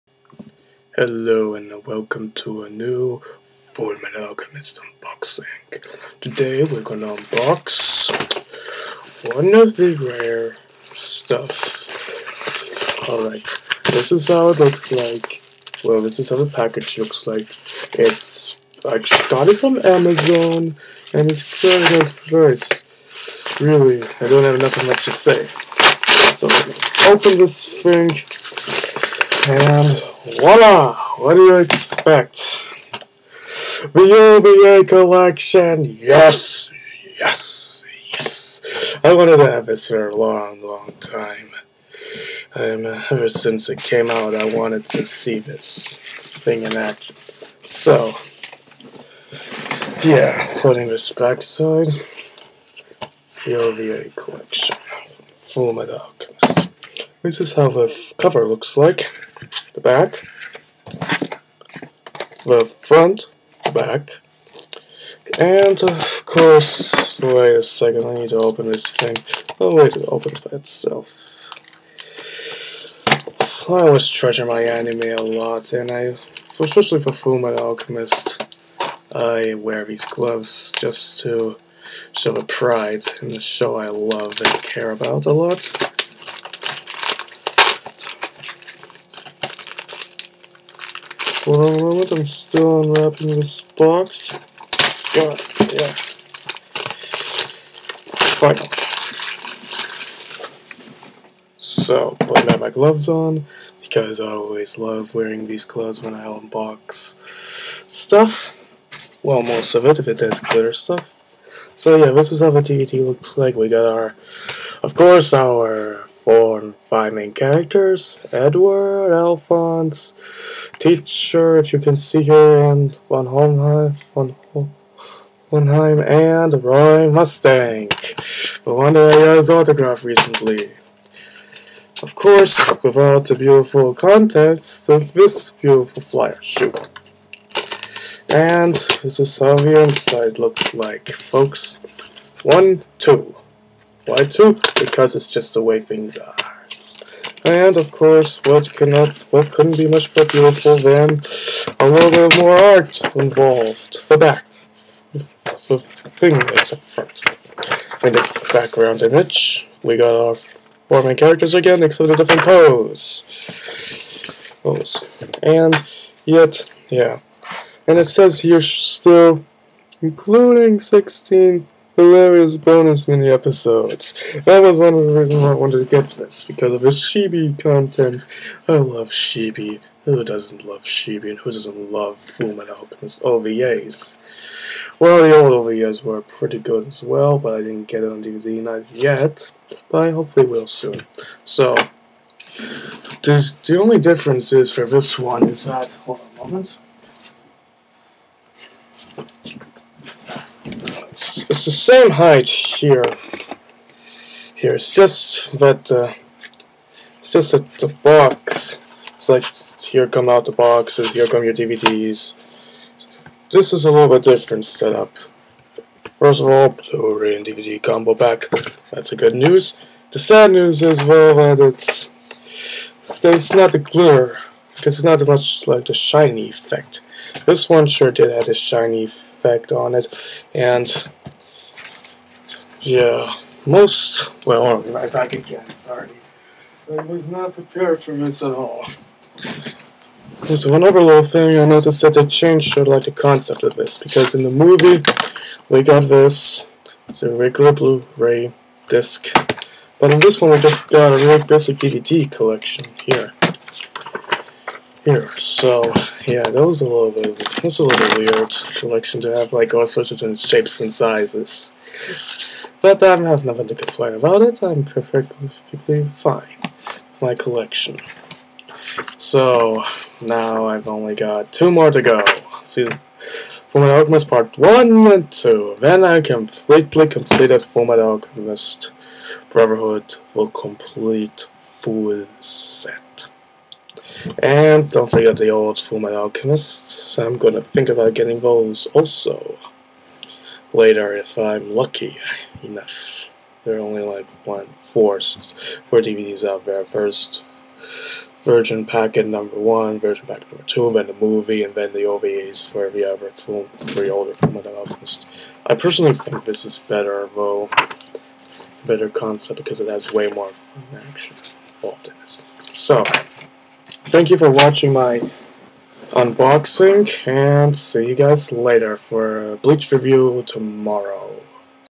Fullmetal Alchemist Brotherhood OVA unboxing
Got it today and it is worth the money.